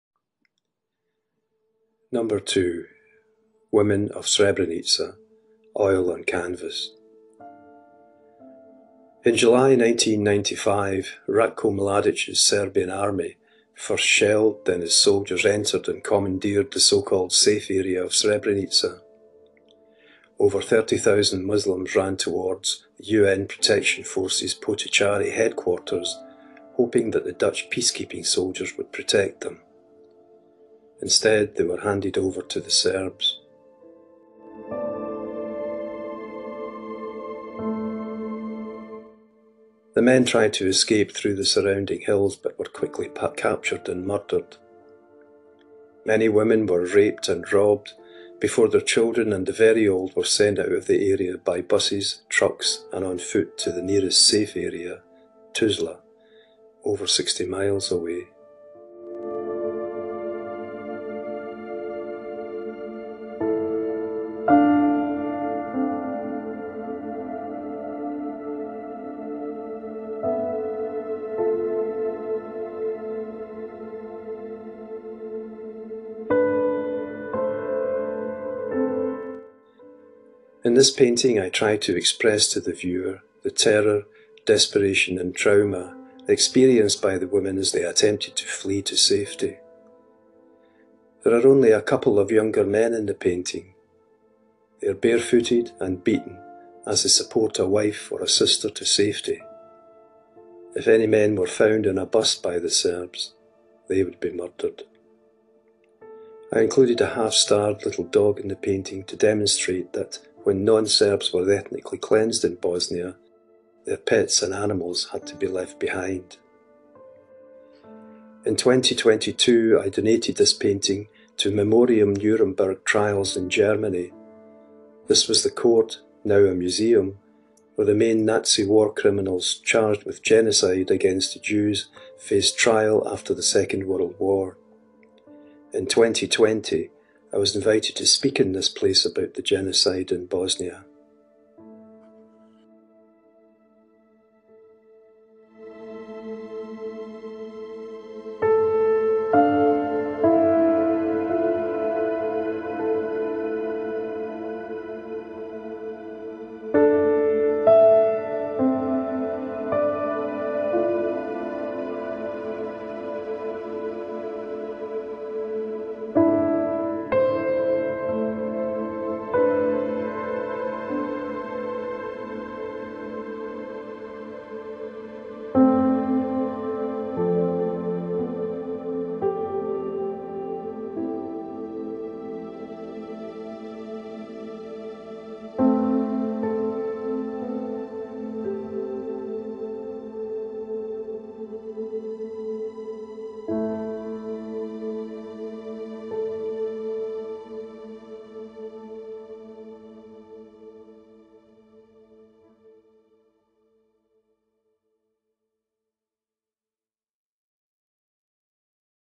Painting of the day: women of srebrenica
Season 1 With  Mr. Robert McNeil, MBE,  FAAPT
Author and Narrator

Our Guest is Mr Robert McNeil, MBE. He is a friend and a tutor before being an artist and forensic technician. he is a Glasgow-based painter who following his retiring from a long career as an anatomical pathology technologist now focuses on his rules as an ambassador for remembering Srebrenica and as an artist for UNESCO's Refugee integration through language and the Arts his artworks include a wide range of subjects including portrays self-portrait still life cityscapes and War.